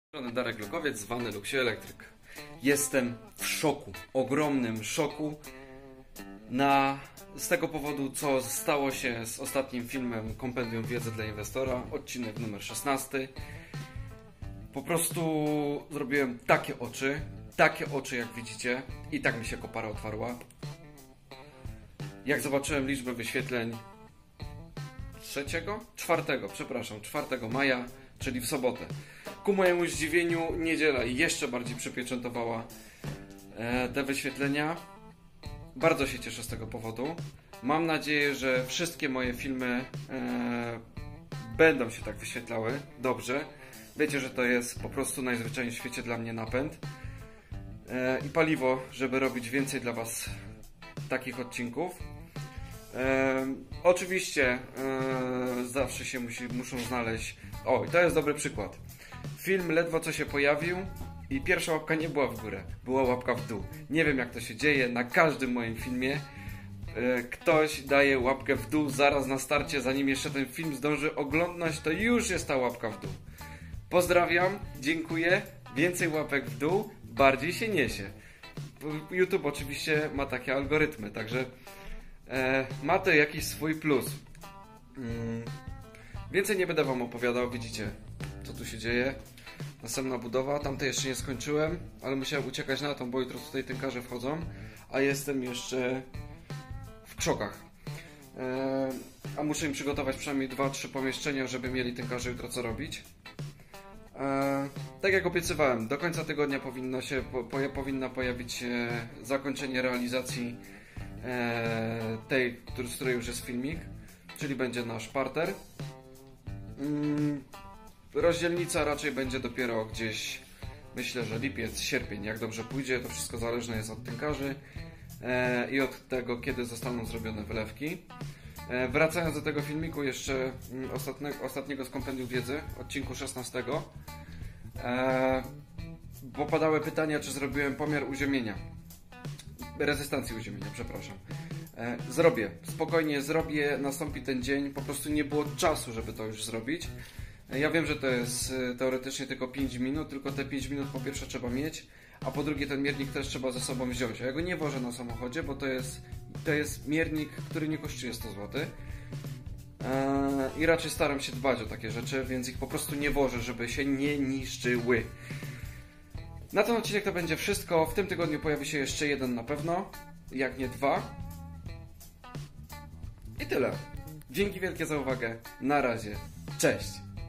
VLOG #15 Jestem w szoku! 12k wyświetleń w 3 dni!
Witajcie!
Jestem w takim szoku, że odbiera mi mowę!
Ilość wyświetleń odcinka KWDI #16 przerosły moje najśmielsze oczekiwania.

Mam nadzieję że ten trend się nie skończy:)

Pozdrawiam